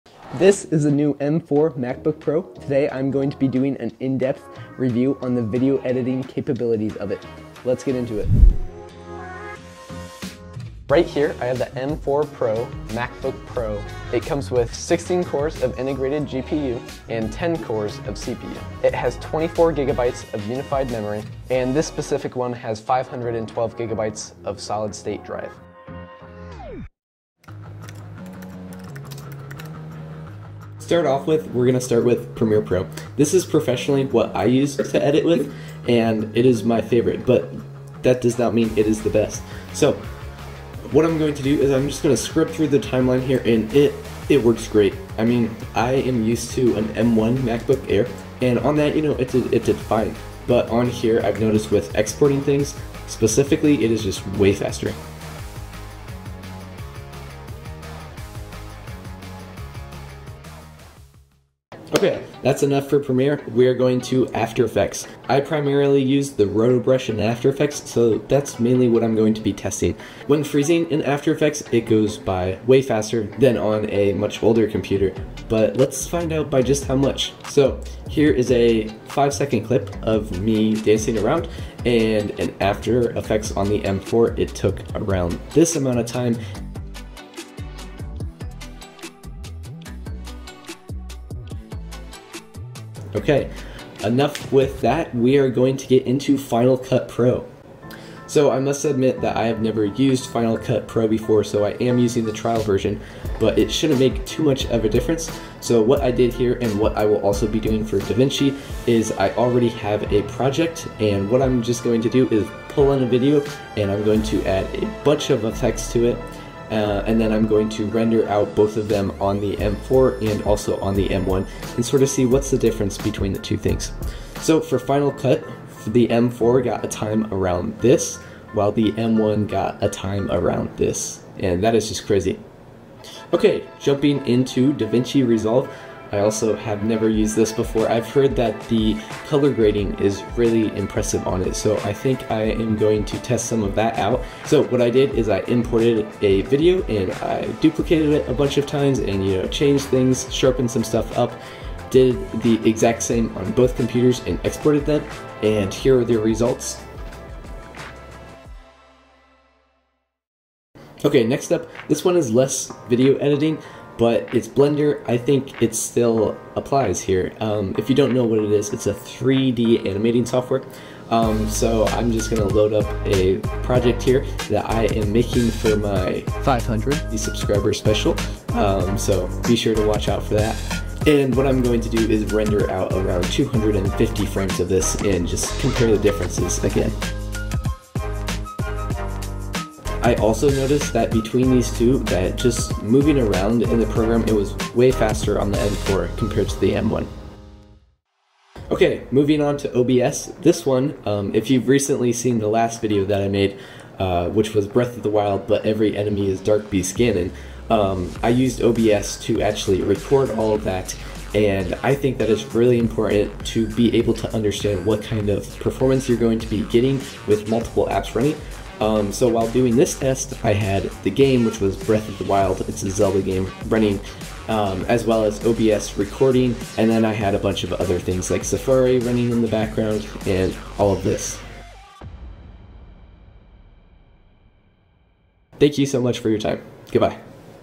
How Good Is the M4 Pro MacBook Pro for Video Editing?
In this video, I test and Compare the new M4 MacBook Pro against the Notorious M1 MacBook Air. I test Premiere Pro, After Effects, DaVinci Resolve, Final Cut Pro, and Blender to see how it handles real-world video editing tasks.

0:00 - Intro
0:11 - Specs
0:27 - Premiere Pro
1:01 - After Effects
1:38 - Final Cut Pro
2:13 - DaVinci Resolve
2:42 - Blender
3:18 - OBS